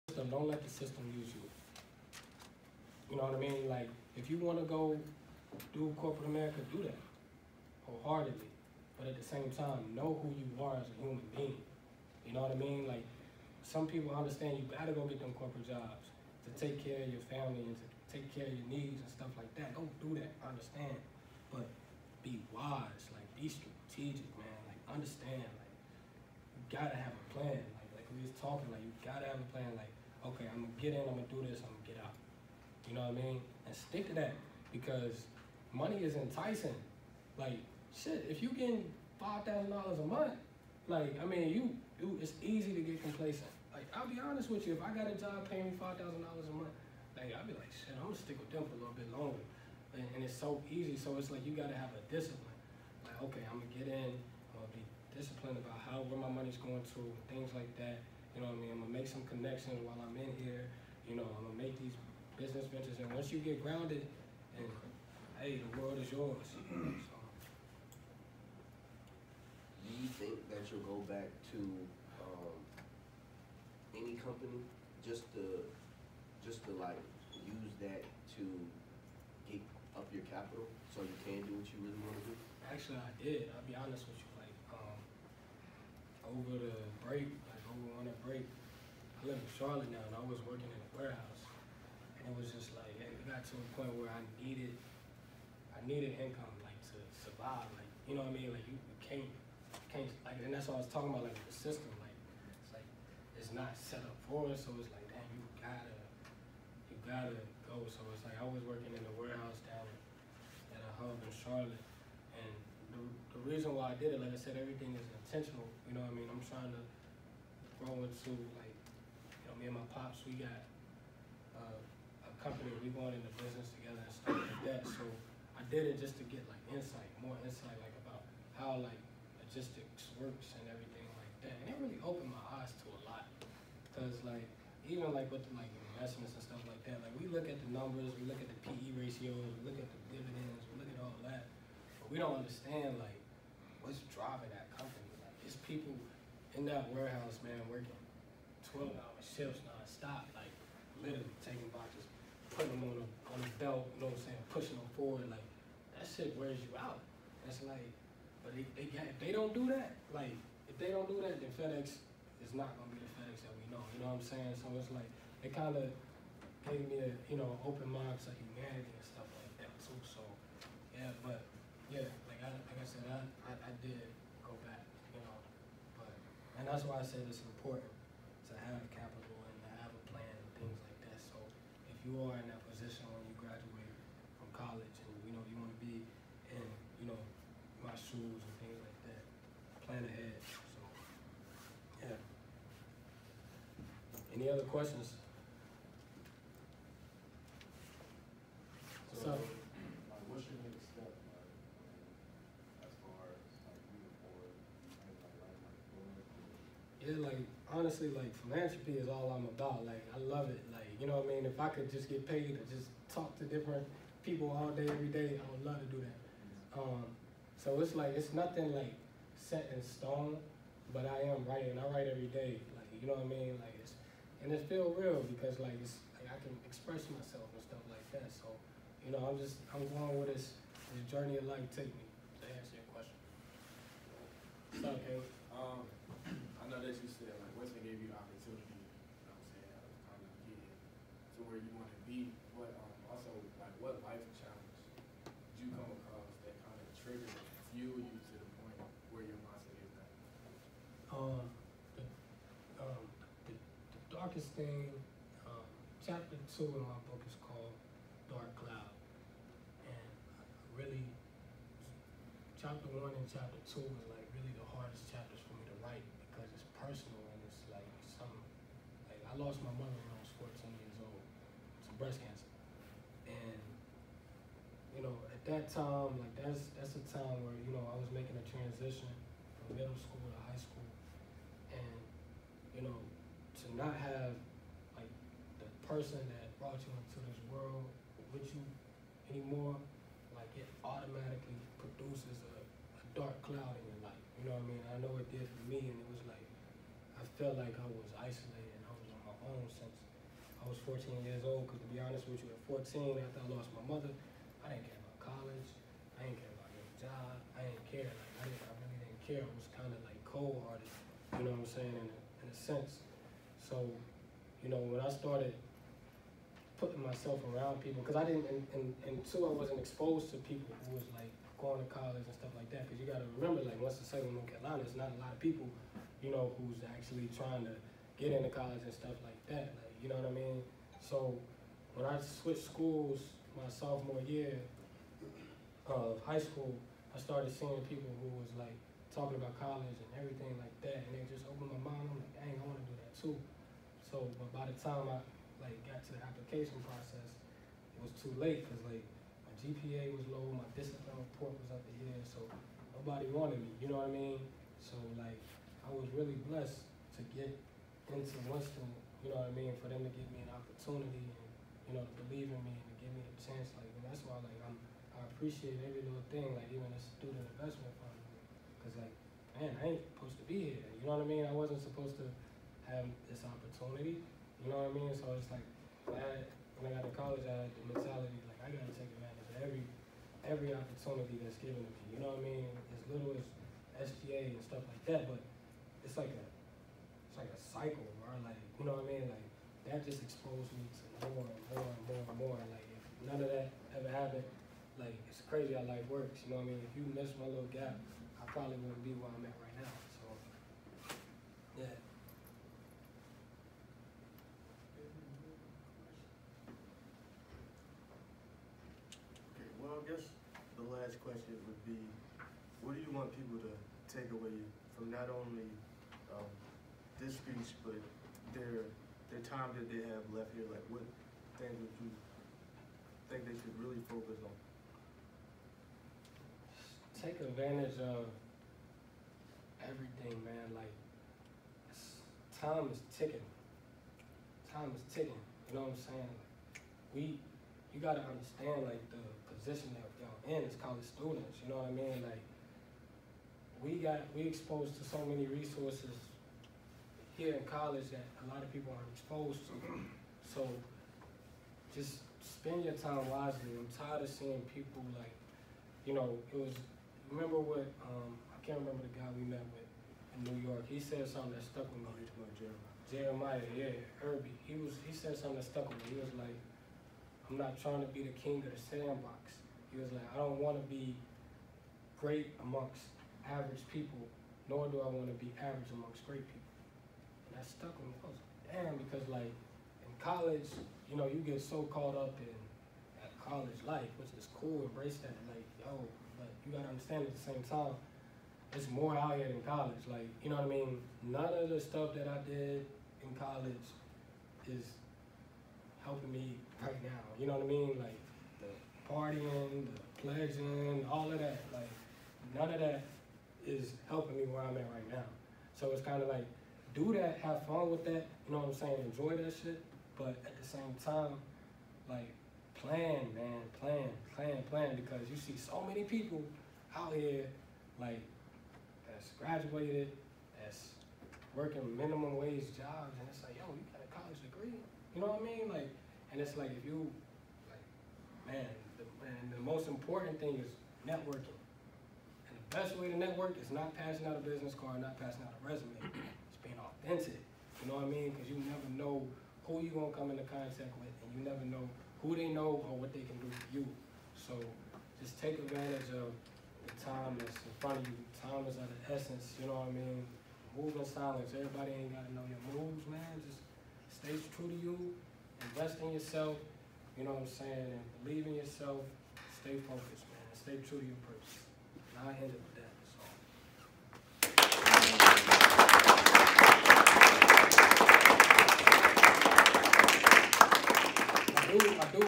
Darrell Q. Slade speaks to Student Investment Fund: Winston-Salem State University
Darrell opens up about his first novel The Alarm Clock